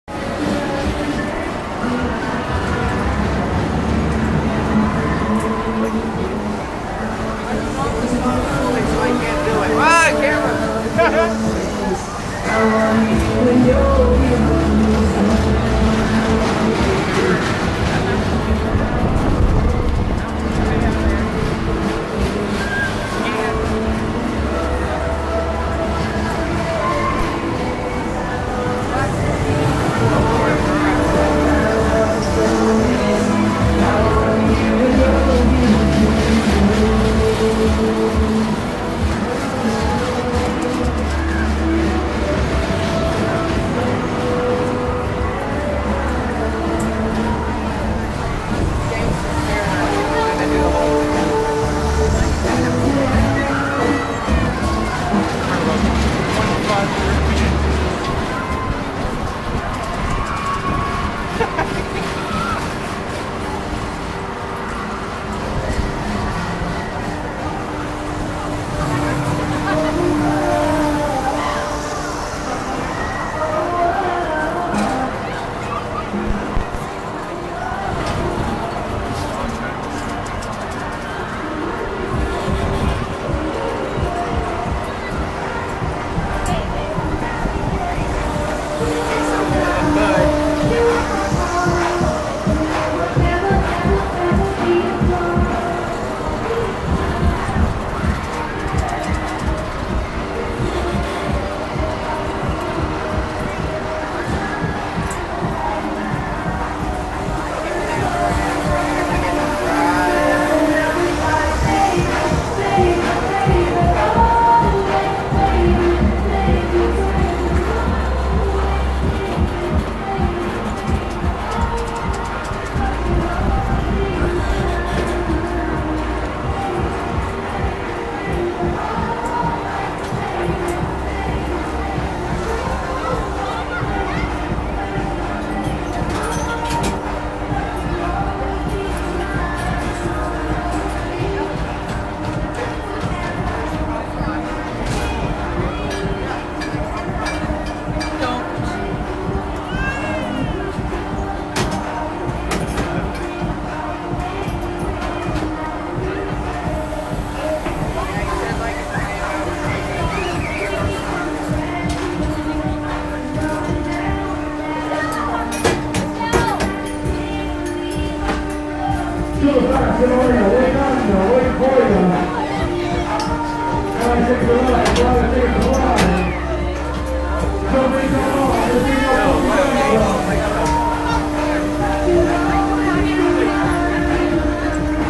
360 Video Riding a Ferris wheel on the Halifax waterfront
360 video of Ferris wheel on the Halifax waterfront